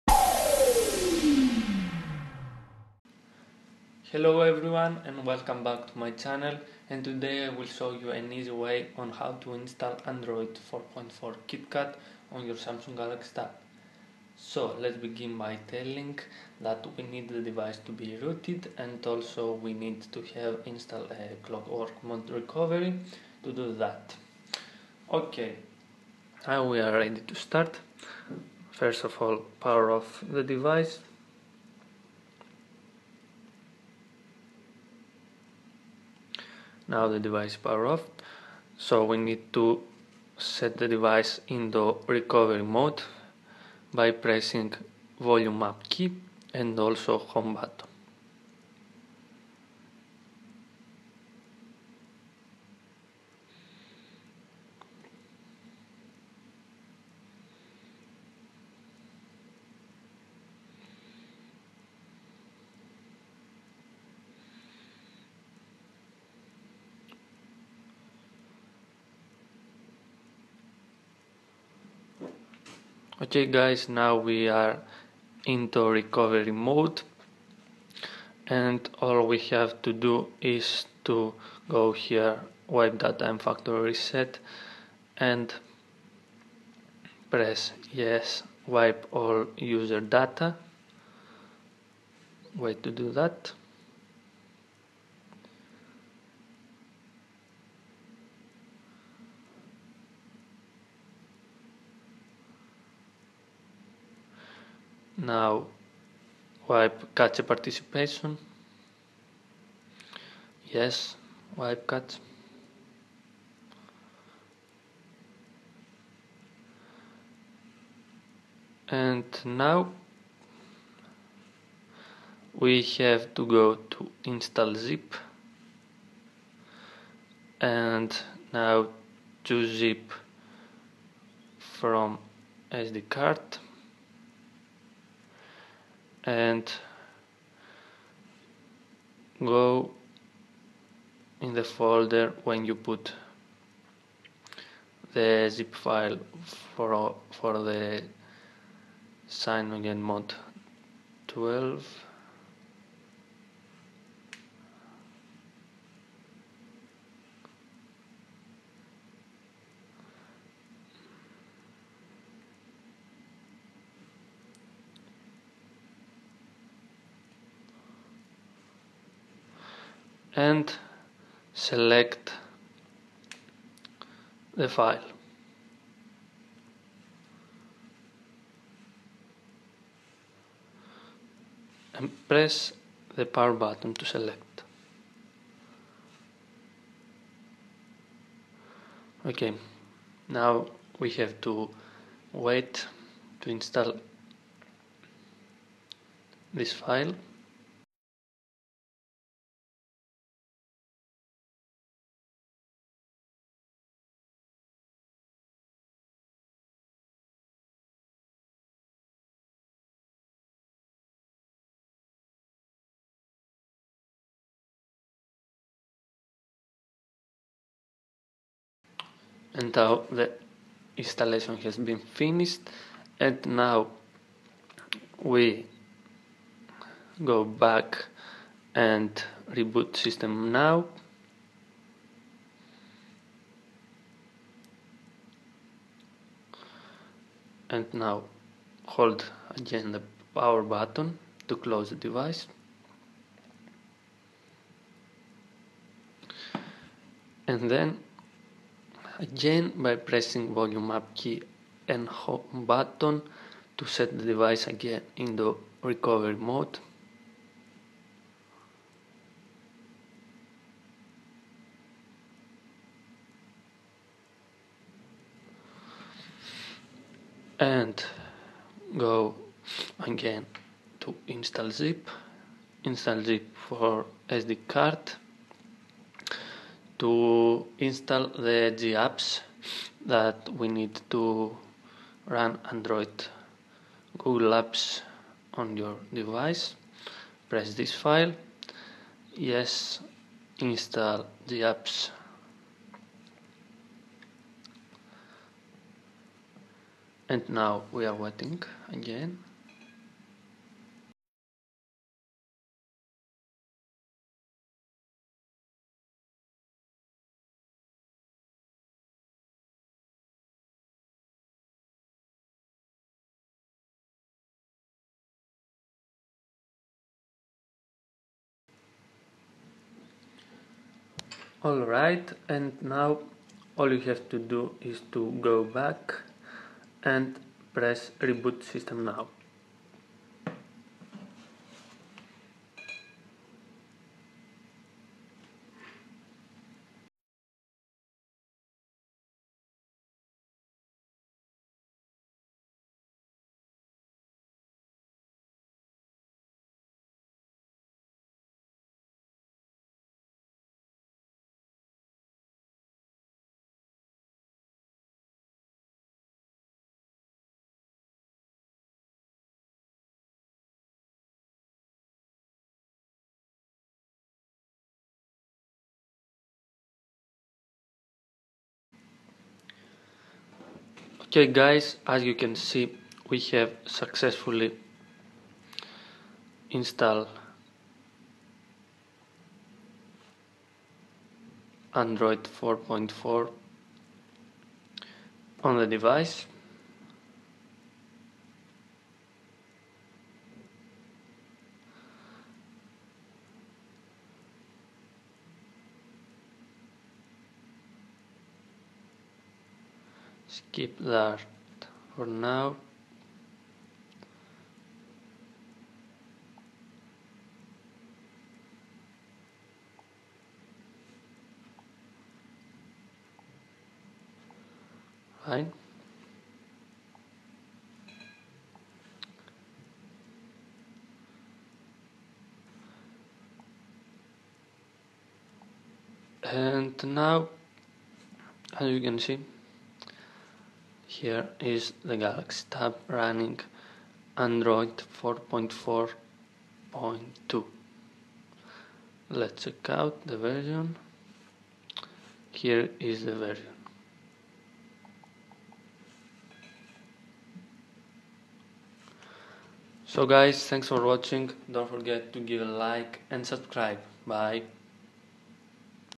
install android 4.4 on galaxy tab p1000 (cm11)(easy way)
This is a guide on how to install cyanogenmod 11(and dont 12 as i tell in video) android kitkat 4.4.2 on the samsung galaxy tab p1000.

(samsung galaxy tab running android 4.4 review)

also (take screenshoot on cm11)

(install flashplayer on android 4.4)

(how to install flappy bird)

NOTE! I'm NOT RESPONSIBLE FOR ANY DAMAGE CAUSED. YOU ARE ON YOUR OWN NOW.

Nick Diamond